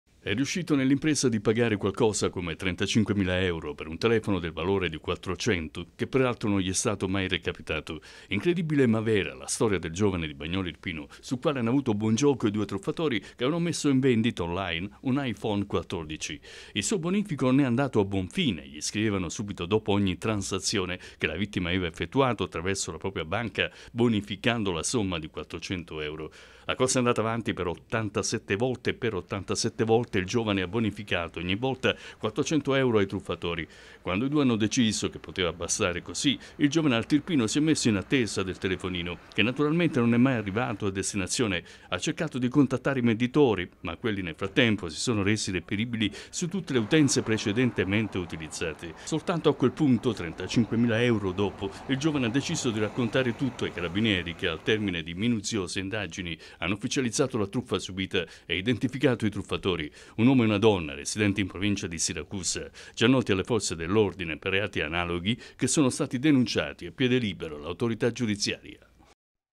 35 mila euro per un Iphone la supertruffa di Bagnoli
Riescono a spillargli 35 mila euro per un IPhone messo in vendita on line. Un giovane di Bagnoli la vittima: ai suoi truffatori ha fatto ben 87 bonifici da 400 euro per un telefono inesistente.